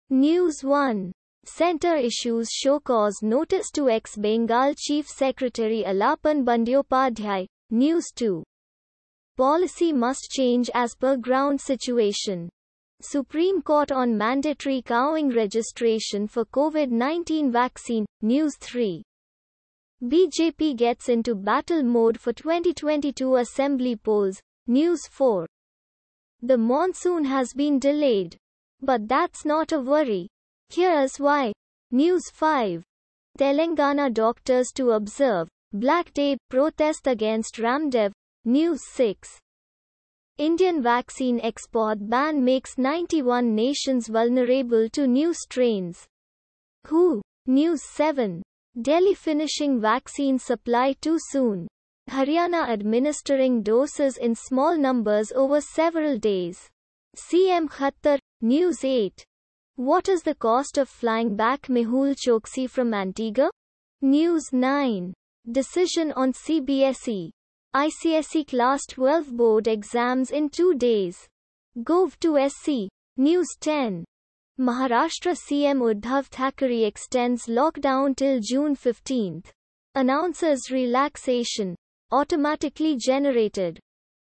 News Headlines (India)- 31/05/2021,11:00 PM
News 1:Centre issues show-cause notice to ex-Bengal chief secretary Alapan Bandyopadhyay, News 2:‘Policy must change as per ground situation’: Supreme Court on mandatory CoWin registration for Covid-19 vaccine, News 3:BJP gets into battle mode for 2022 Assembly polls, News 4:The monsoon has been delayed, but that’s not a worry. Here’s why, News 5:Telangana doctors to observe ‘Black Day’ protest against Ramdev, News 6:Indian Vaccine Export Ban Makes 91 Nations Vulnerable To New Strains: WHO, News 7:Delhi finishing vaccine supply too soon; Haryana administering doses in small numbers over several days: CM Khattar, News 8:What Is The Cost Of Flying Back Mehul Choksi From Antigua?, News 9:Decision on CBSE, ICSE Class 12 Board Exams in 2 Days: Govt to SC, News 10:Maharashtra CM Uddhav Thackeray extends lockdown till June 15, announces relaxation. Automatically Generated For Advertisement Contact